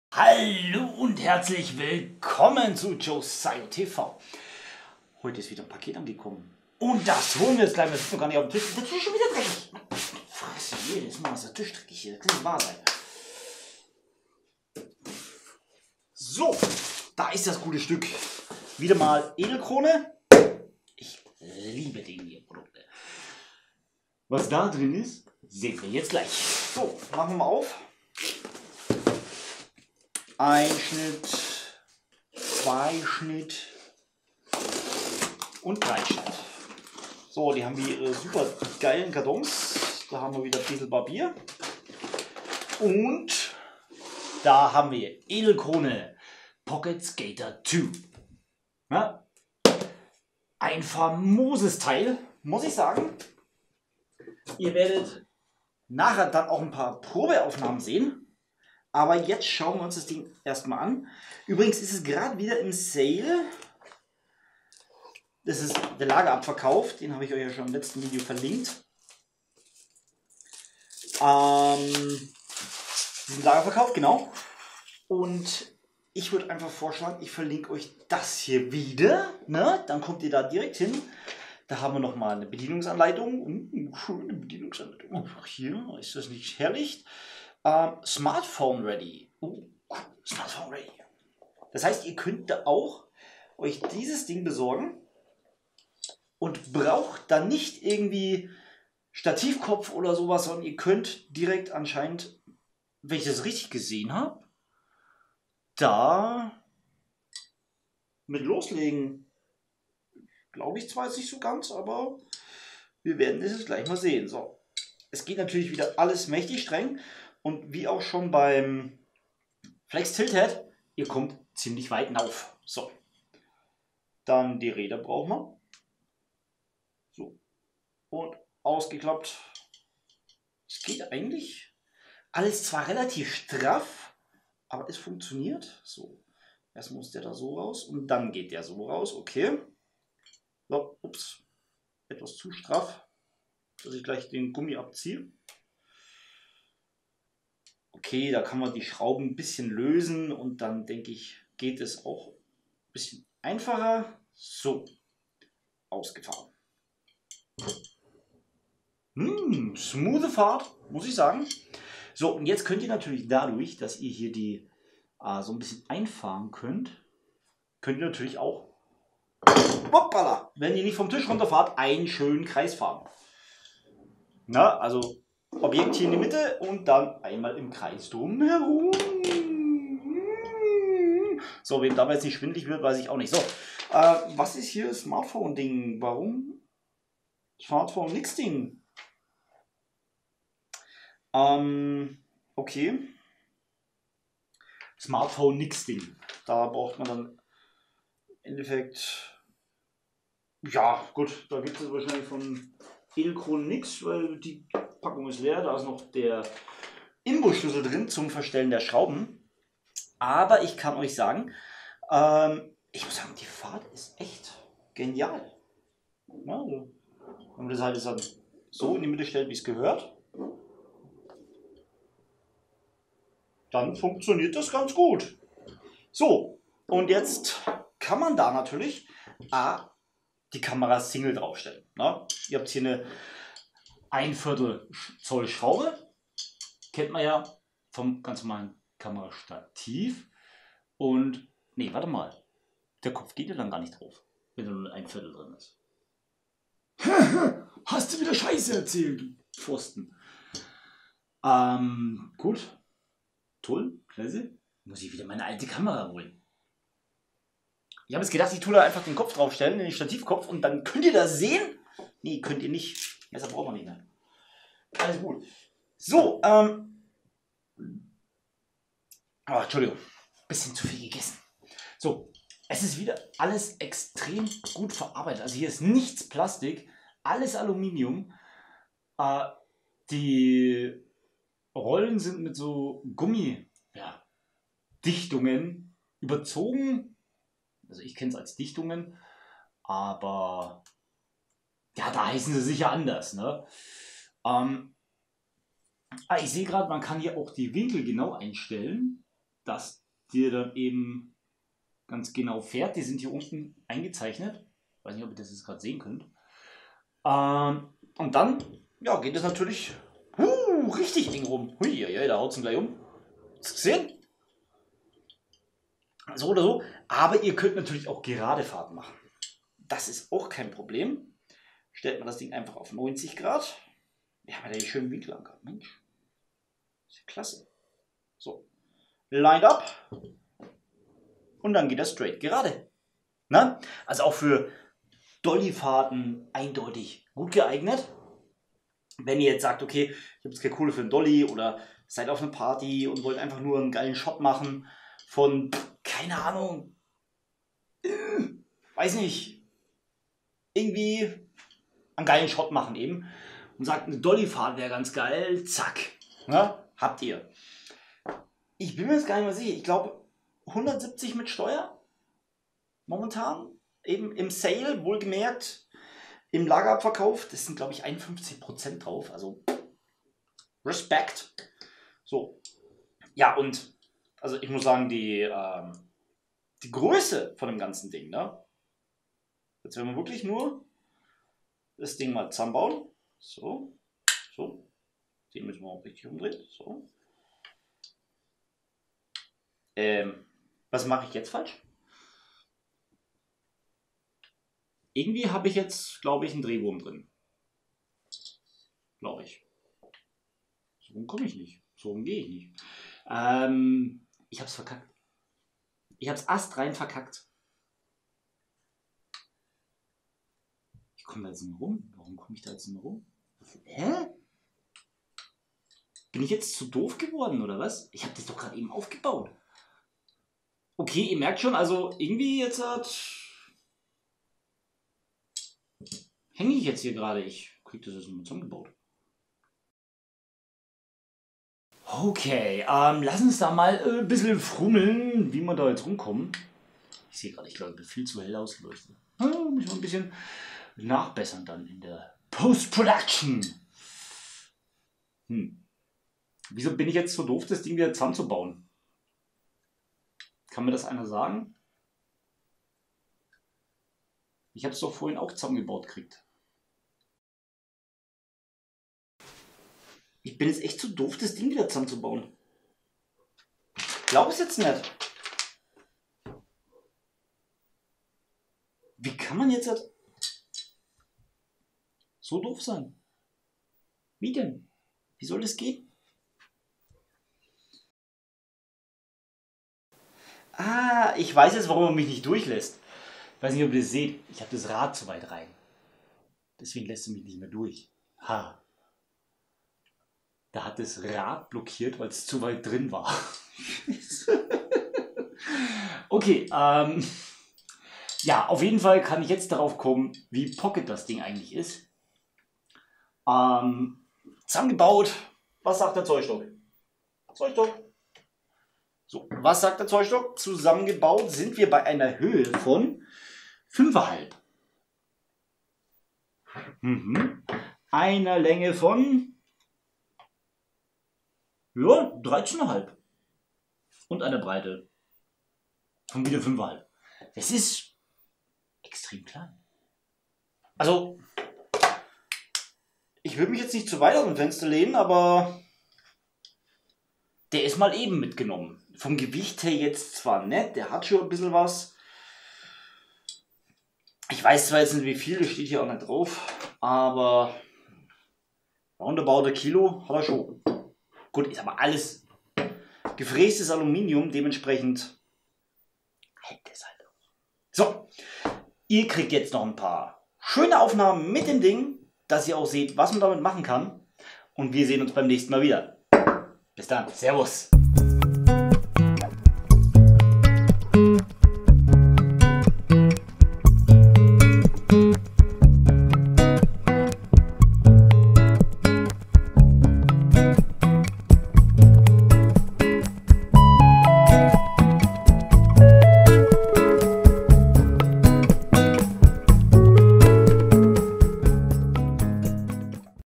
Edelkrone PocketSkater 2
Der PocketSKATER 2 von Edelkrone ist eingetroffen und sollte mir fast vom Tisch.